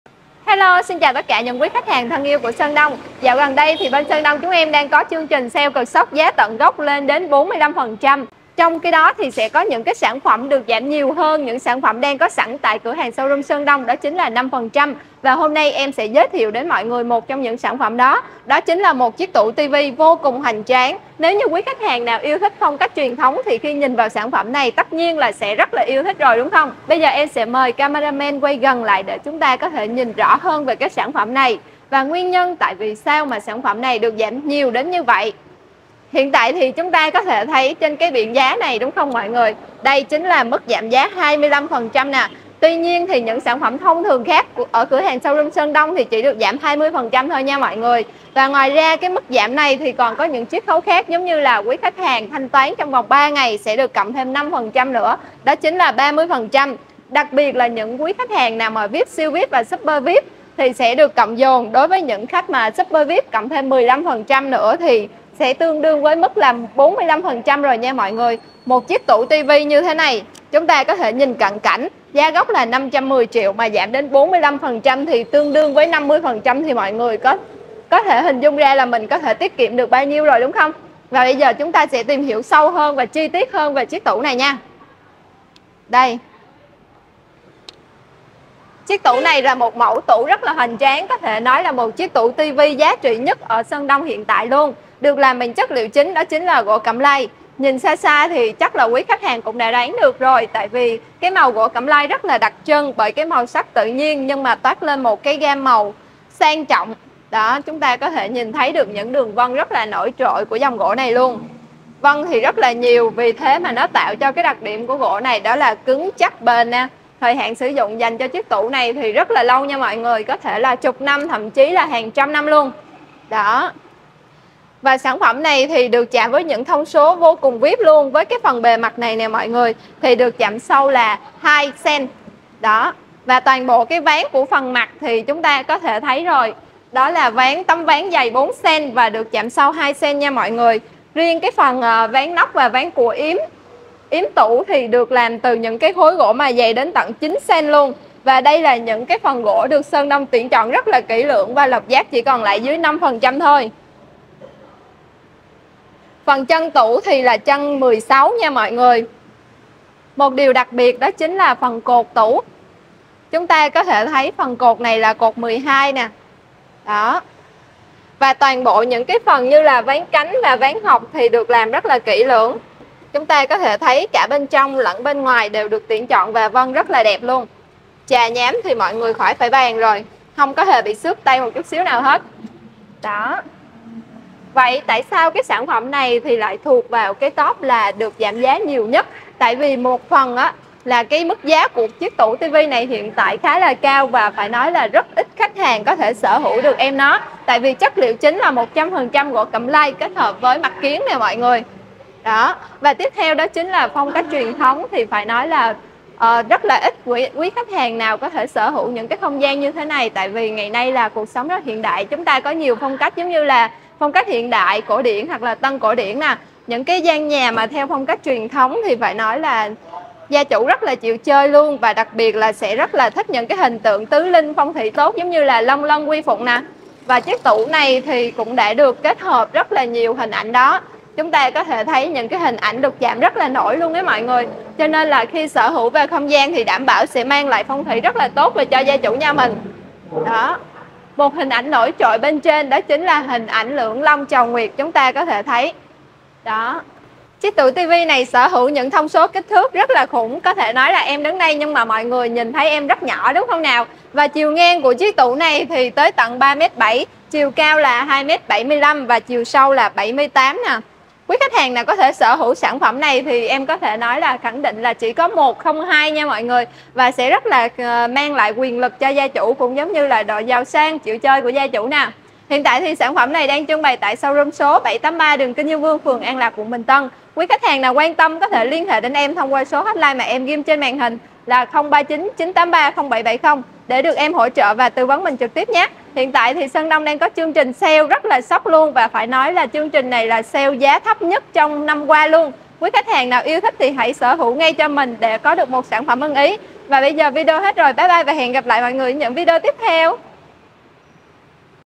CHẤN ĐỘNG| Giảm sâu hơn 150 triệu cho mẫu tủ tivi cẩm lai hoành tráng số 1 này
Địa chỉ: 783 - Kinh Dương Vương - An Lạc - Bình Tân
Tủ rượu cẩm lai lớn Tứ Linh hội ngộ VIP – TRAS01
• Chất liệu: Gỗ cẩm lai (xuất sứ từ campuchia và Việt Nam)
• Tổng kích thước tủ tivi và 2 tủ rượu : cao 2m75, sâu 78cm, ngang 3m7
Bảo hành 5 năm – Bảo trì vĩnh viễn